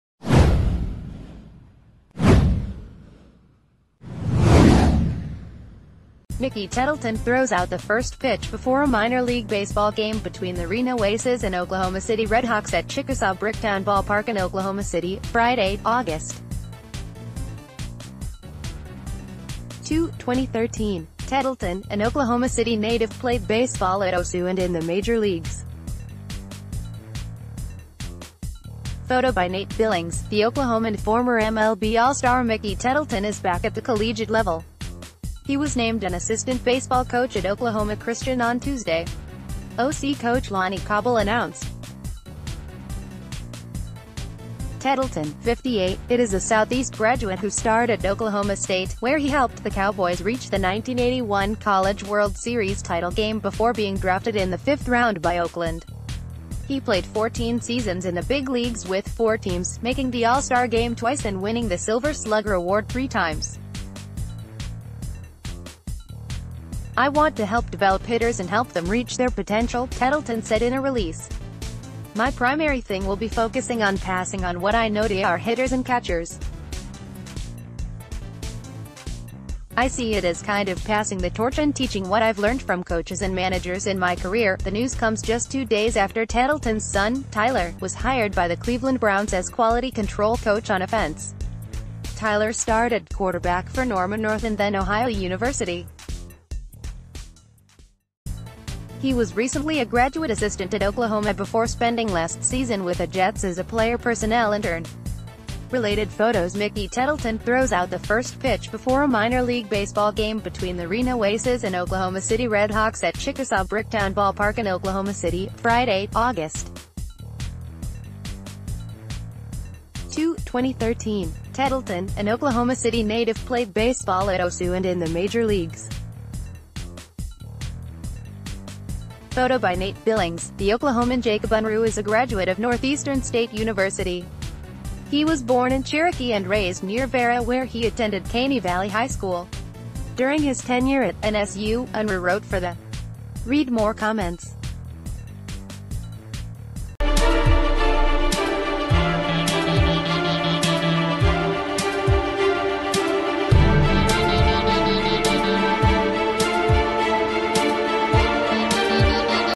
Former MLB All-Star Mickey Tettleton joins Oklahoma Christian staff
Mickey Tettleton throws out the first pitch before a minor league baseball game between the Reno Aces and Oklahoma City RedHawks at Chickasaw Bricktown Ballpark in Oklahoma City, Friday, Aug. 2, 2013. Tettleton, an Oklahoma City native, played baseball at OSU and in the major leagues. Photo by Nate Billings, The Oklahoman Former MLB All-Star Mickey Tettleton is back at the collegiate level. He was named an assistant baseball coach at Oklahoma Christian on Tuesday. OC coach Lonny Cobble announced...